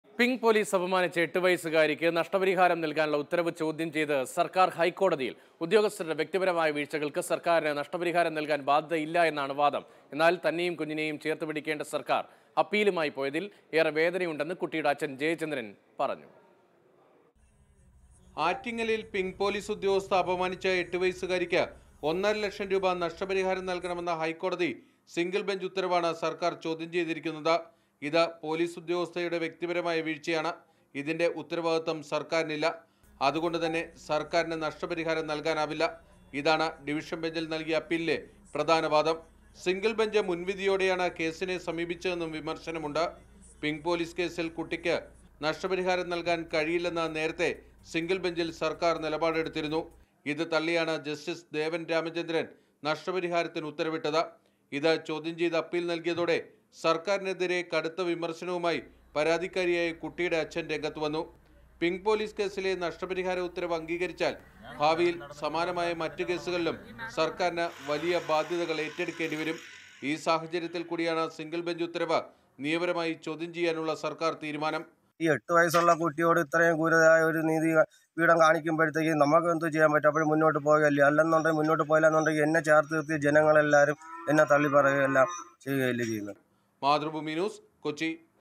പിങ്ക് പോലീസ് സംഭവം: ചേർത്ത് പിടിക്കേണ്ട സർക്കാർ അപ്പീലുമായി പോയതിൽ വേദനയുണ്ടെന്ന് ജയചന്ദ്രൻ
ചേർത്ത് പിടിക്കേണ്ട സർക്കാർ അപ്പീലുമായി പോയതിൽ വേദനയുണ്ടെന്ന് പിങ്ക് പോലീസ് അപമാനിച്ച കുട്ടിയുടെ അച്ഛൻ ജയചന്ദ്രൻ പ്രതികരിച്ചു.

Happy viewing!